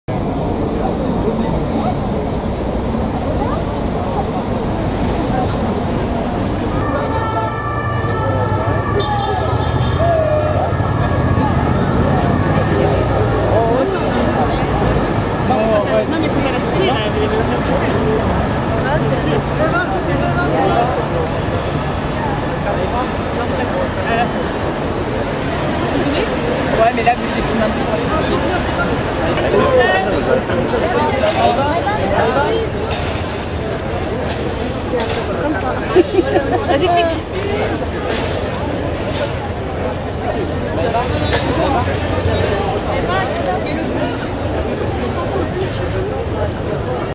Miss France 2018 Maëva Coucke sur le tapis rouge / avant premiere L'Ombre d'Emily 18 septembre 2018
Miss France 2018 Maëva Coucke sur le tapis rouge de l'avant premiere du film L'Ombre d'Emily 18 septembre 2018 @ Paris UGC Normandie Champs Elysées